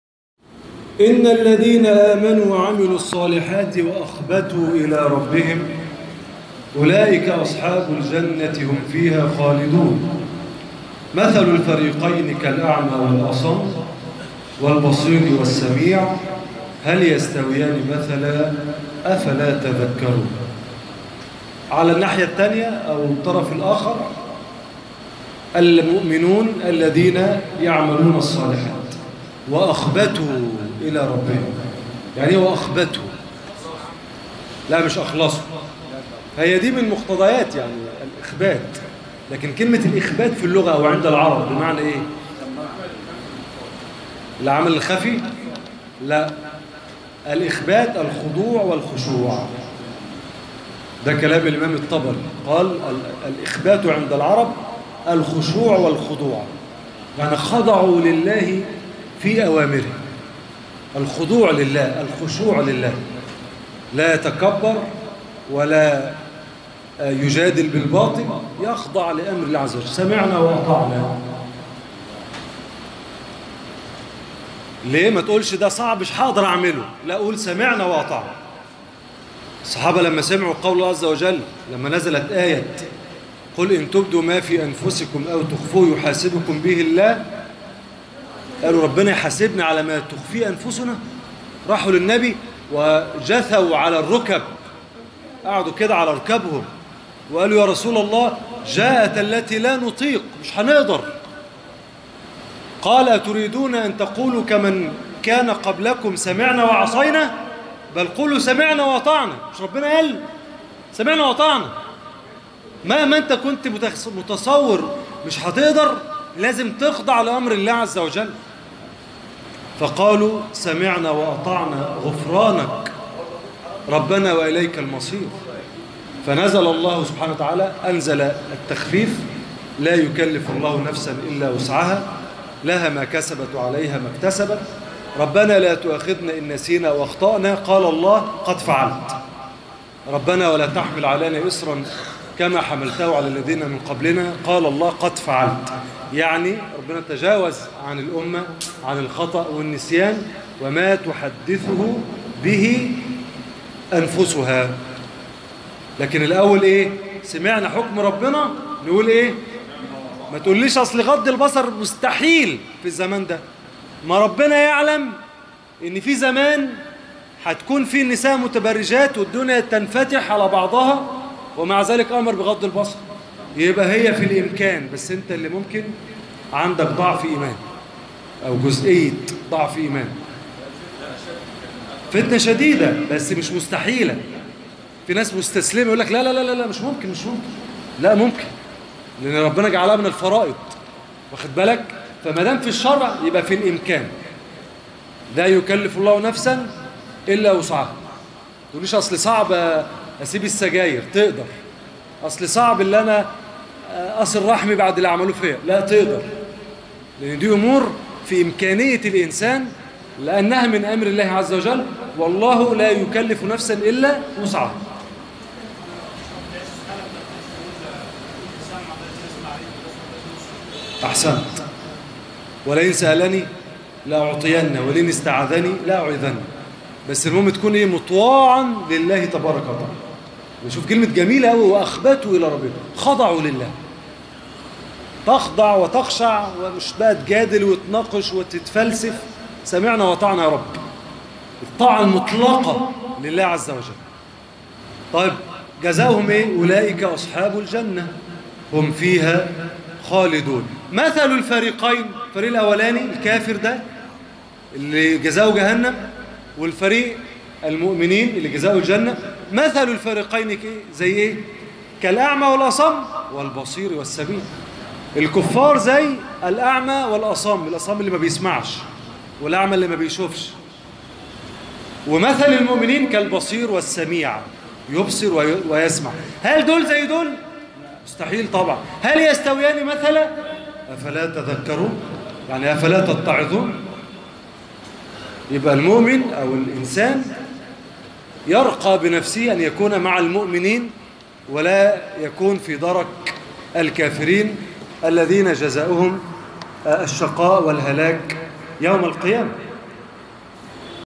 التفسير المبسط لسورة هود (23 ، 24) مثل الفريقين كالأعمى والأصم والبصير والسميع | د.أحمد رجب
{ إِنَّ ٱلَّذِینَ ءَامَنُوا۟ وَعَمِلُوا۟ ٱلصَّـٰلِحَـٰتِ وَأَخۡبَتُوۤا۟ إِلَىٰ رَبِّهِمۡ أُو۟لَـٰۤىِٕكَ أَصۡحَـٰبُ ٱلۡجَنَّةِۖ هُمۡ فِیهَا خَـٰلِدُونَ (٢٣) ۞ مَثَلُ ٱلۡفَرِیقَیۡنِ كَٱلۡأَعۡمَىٰ وَٱلۡأَصَمِّ وَٱلۡبَصِیرِ وَٱلسَّمِیعِۚ هَلۡ یَسۡتَوِیَانِ مَثَلًاۚ أَفَلَا تَذَكَّرُونَ (٢٤) }
[سُورَةُ هُودٍ: ٢٣-٢٤]